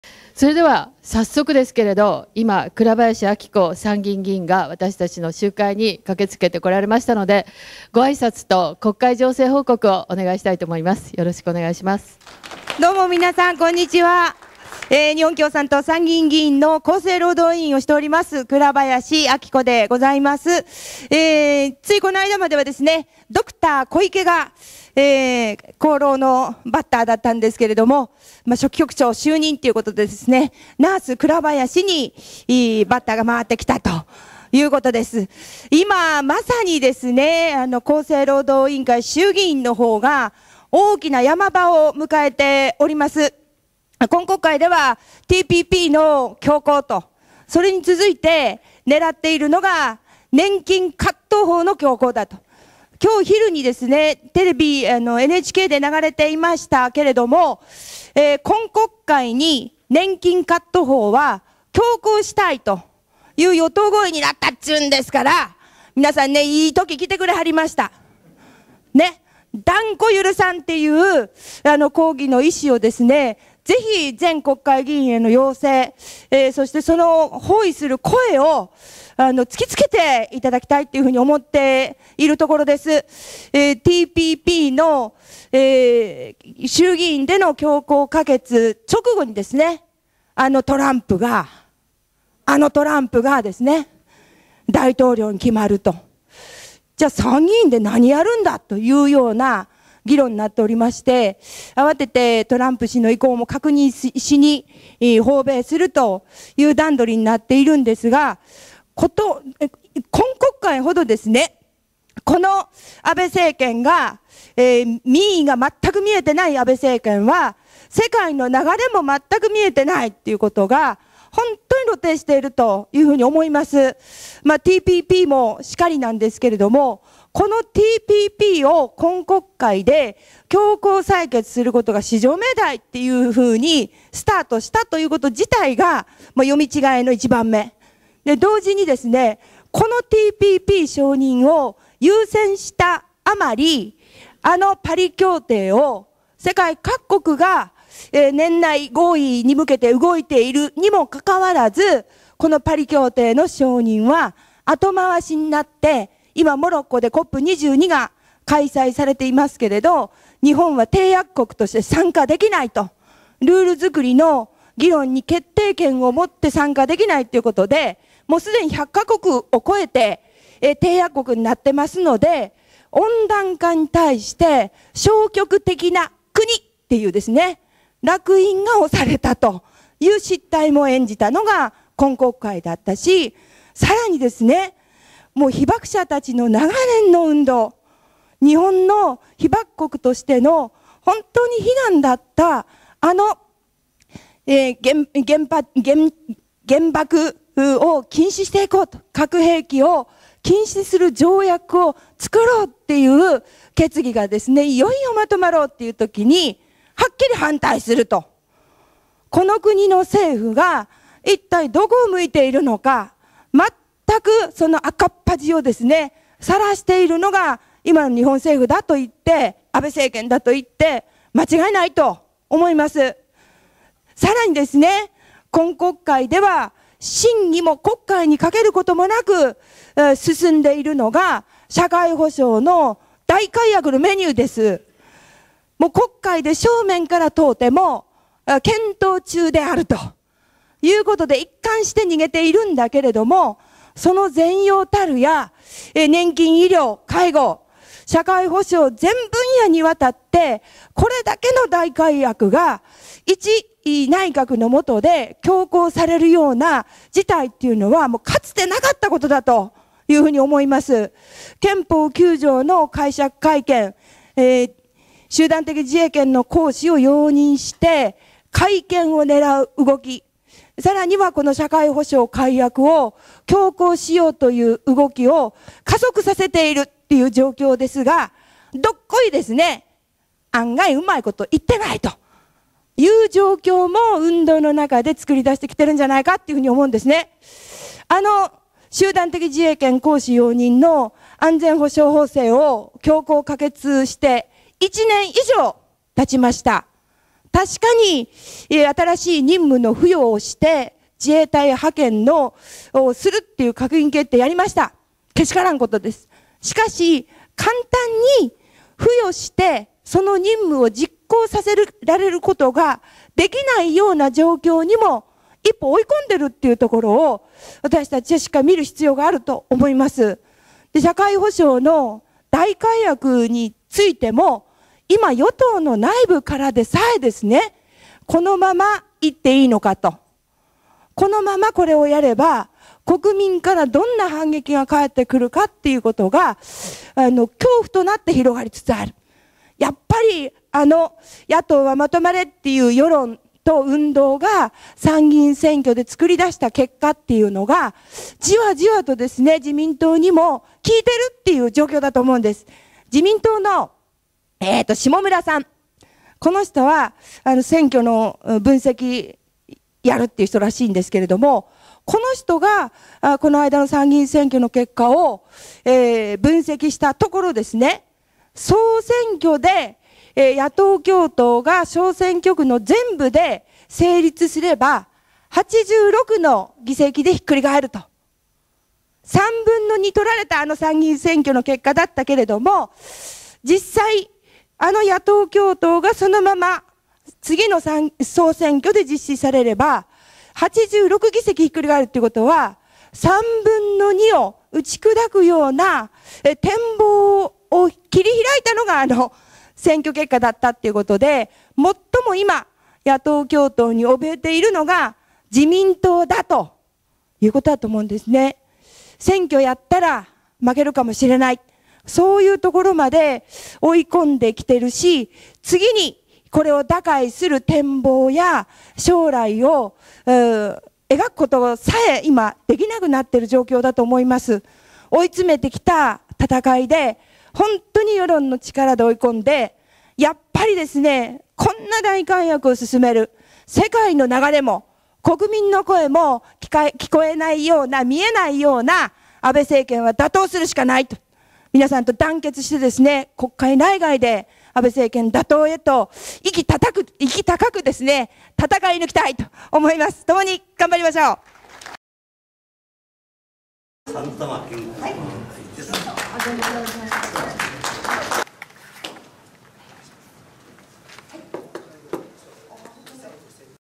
倉林明子参院議員 国会情勢報告
１１月１６日、「安全・安心の医療、介護を」中央社会保障協議会国会行動　院内集会